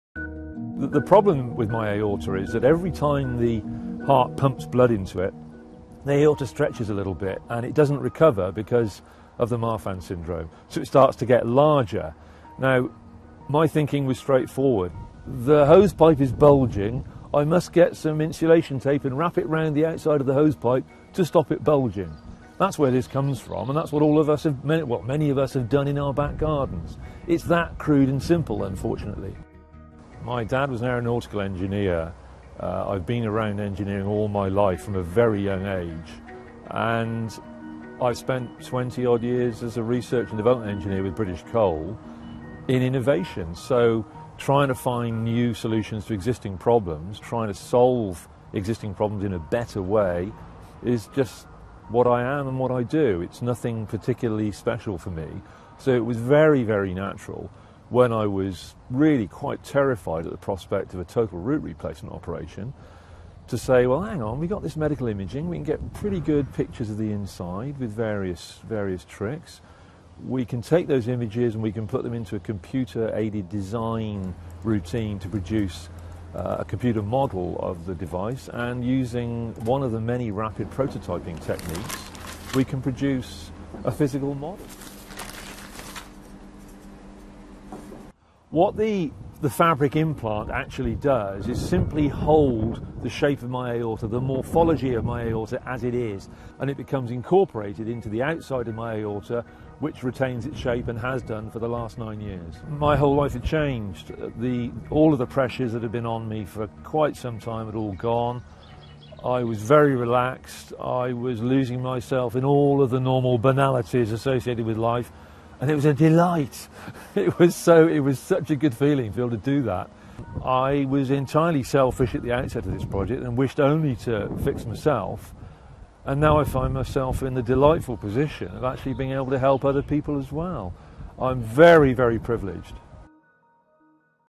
ExoVasc® Personalised External Aortic Root Support
Tal Golesworthy, born in 1956, in UK, is an engineer who was diagnosed with Marfan Syndrome. He developed ExoVasc, his own personalized external aortic root support, to prevent aorta rupture.

RIGHTS NOT OWNED.